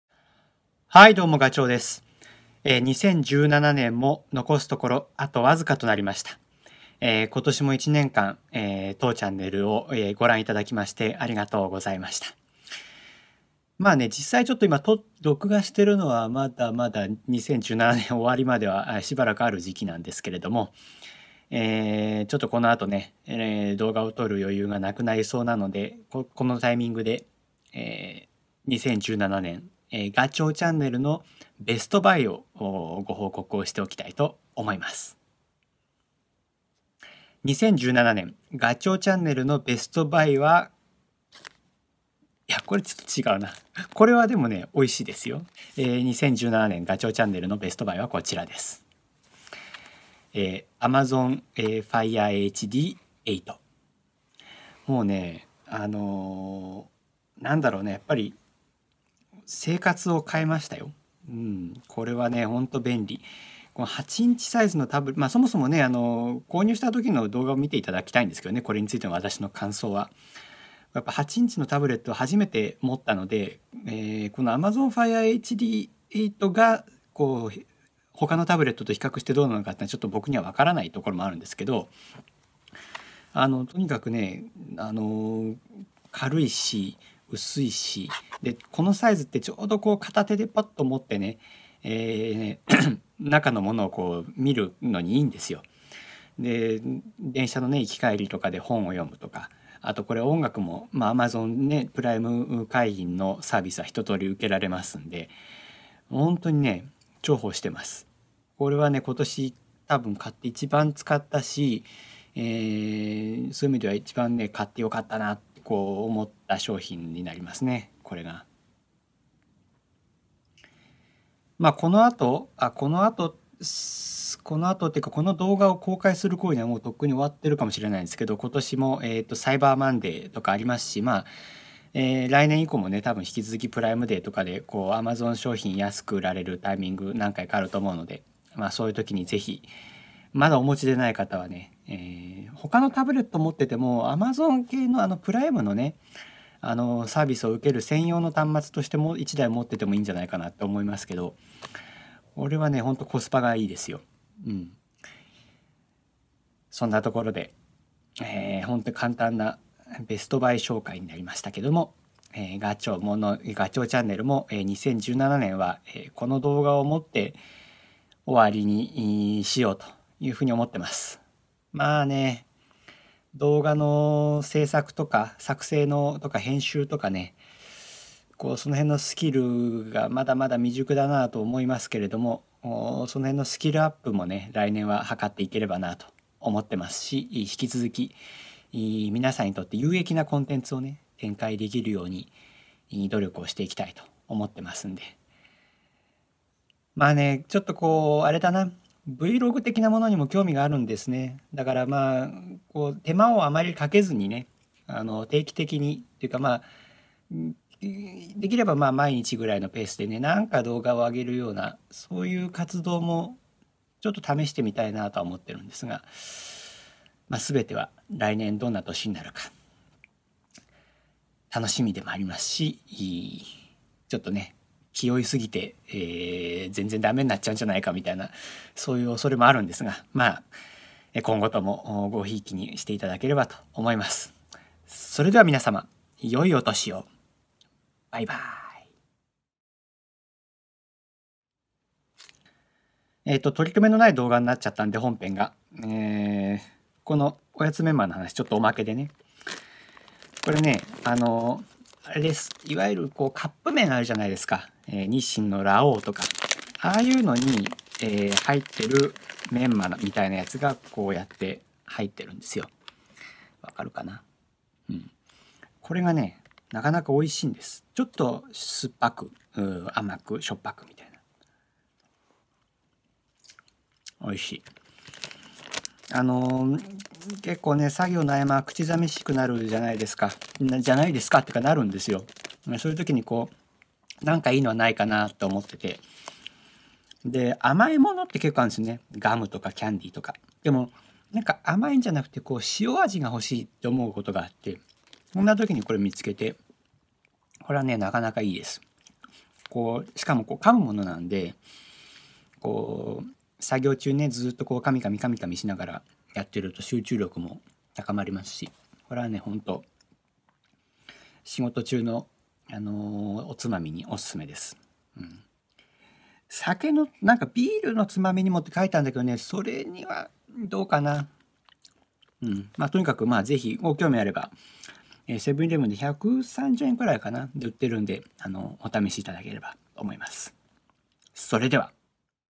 2017年ベストバイ雑談
2017年、ガチョウチャンネルのベストバイは...これだっ！

いやまあ特別珍しいものでもなんでもないんですけれどね。でもそういうものだからこそ、手に入れてから、なるほどみんながもてはやすのわかるっ！と納得したんでありました。

動画としては雑談...むしろおやつメンマこそ、知らない人にオススメしたい一品（笑）好みはかなり分かれそうですが;;